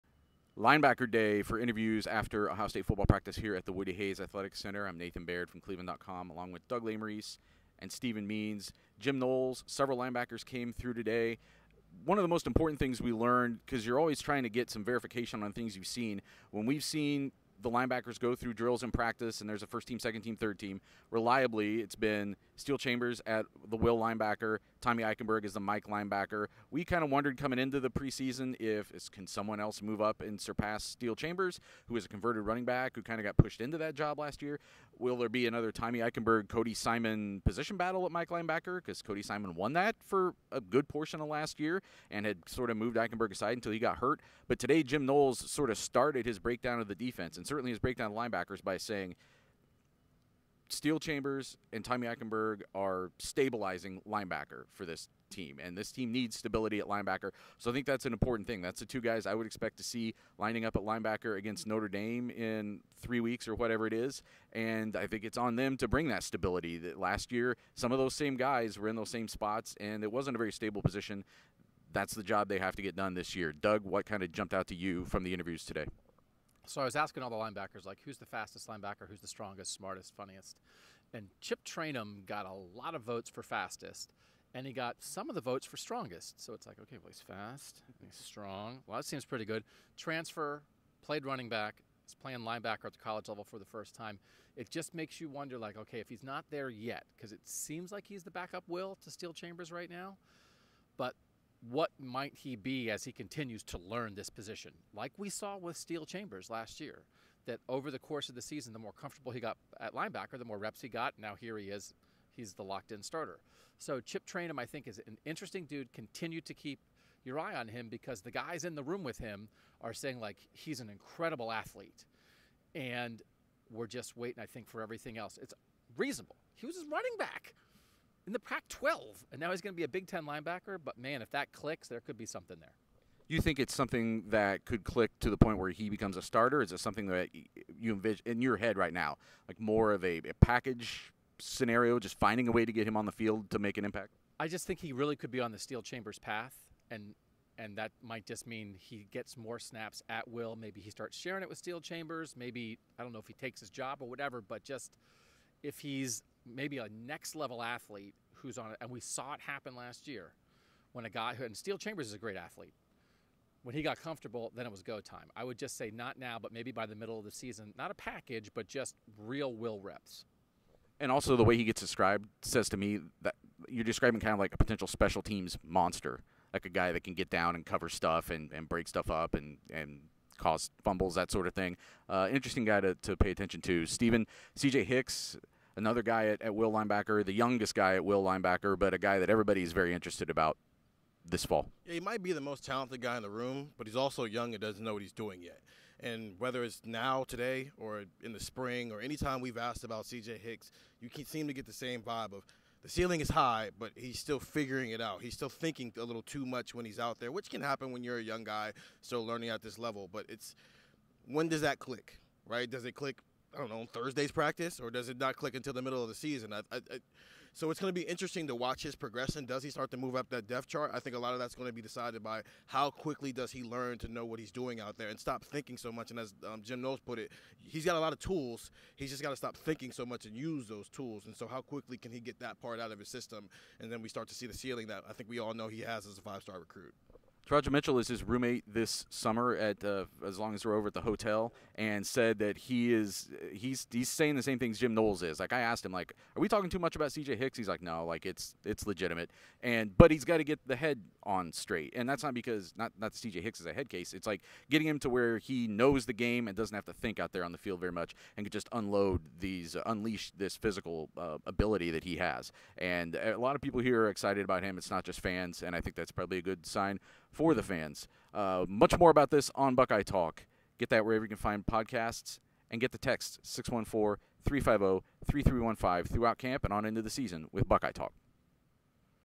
What’s the most interesting thing about Ohio State football’s linebackers?
Stephen Means, Nathan Baird and Doug Lesmerises discuss Ohio State football's linebackers.